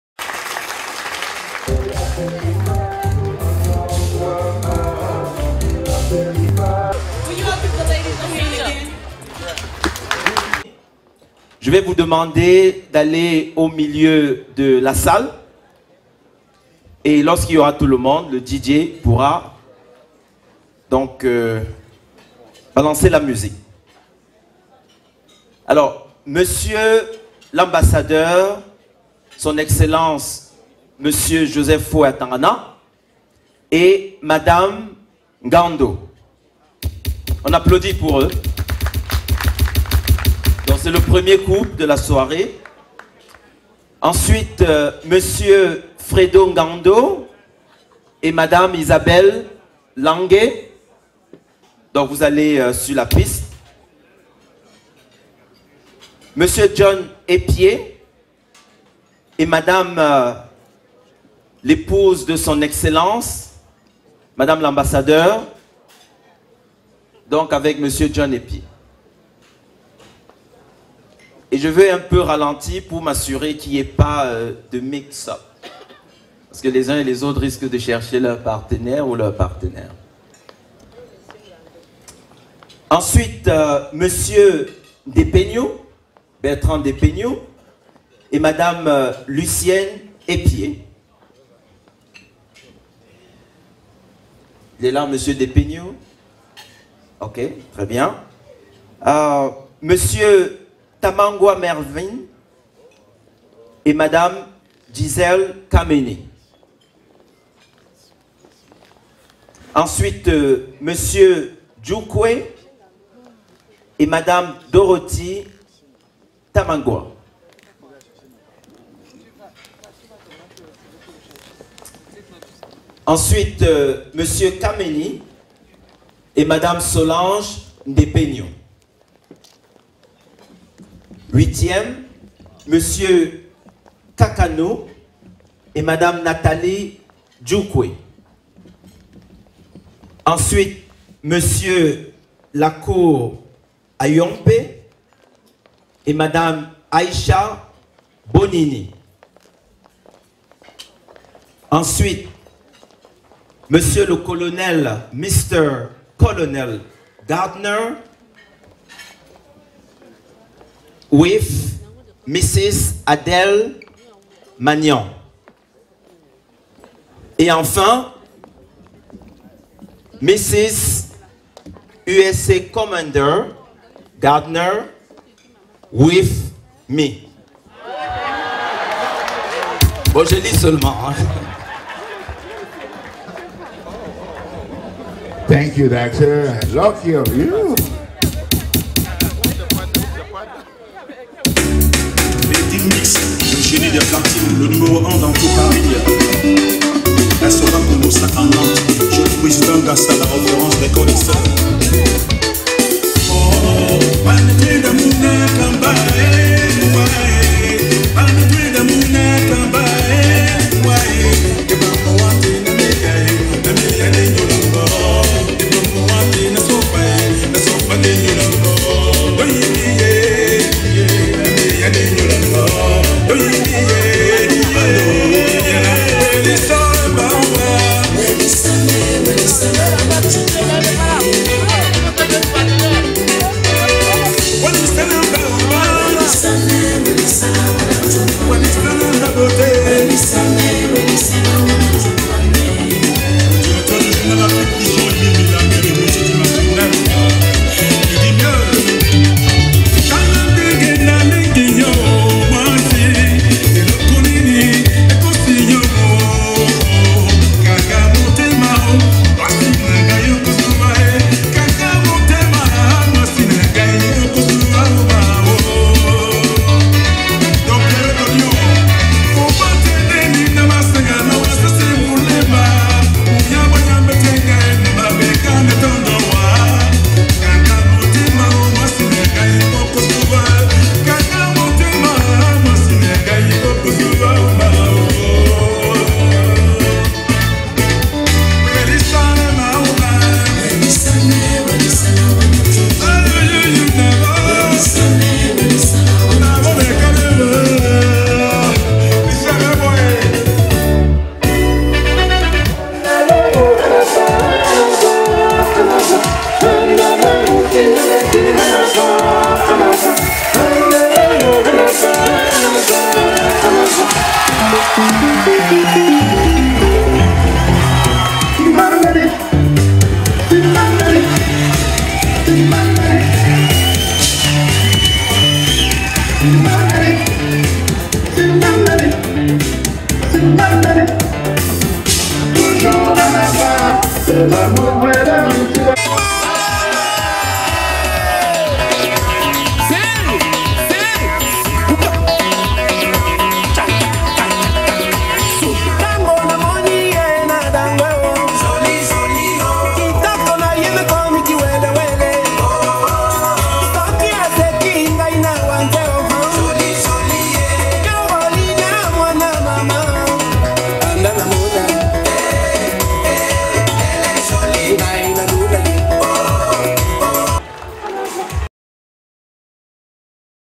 La Diaspora USA à l'honneur avec les Smart Ladies
Gala annuel des Smart Ladies in Washington DC en présence de son Excellence l'Ambassadeur du Cameroun aux Etats Unis Joseph Foe Atangana. Docta Samuel Dongmo présentant le tour d'honneur.

Smart Ladies Annual Gala in Washington DC in the presence of His Excellency the Ambassador of Cameroon to the United States Joseph Foe Atangana. Docta Samuel Dongmo presenting the tour d’honneur